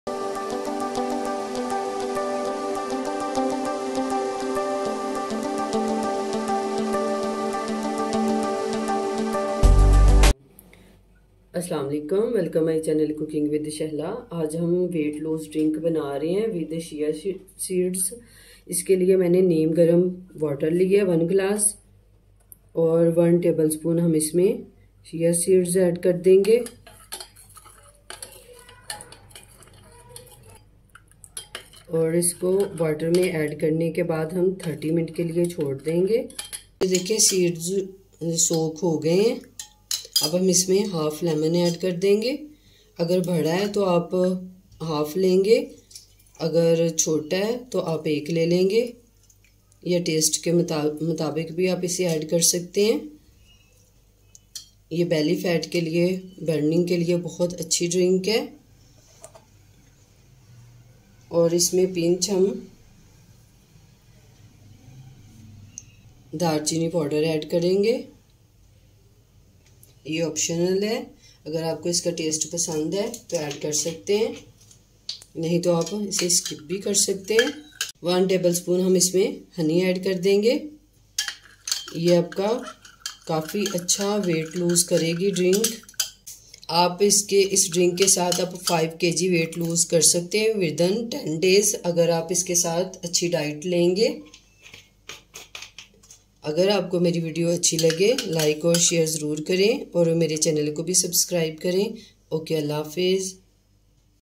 Chia Seed with lemon juice Morning Drink for weight loss!10 kg weight lose in 5 days
This recipe especially make for those persons who want to lose their weight immediately you can you can reduce your weight up to 10 kg within 5 days magic so now start to take drink in morning for best results of about your weight please watch this video and make this Shia seat drink with lemon and enjoy your slimness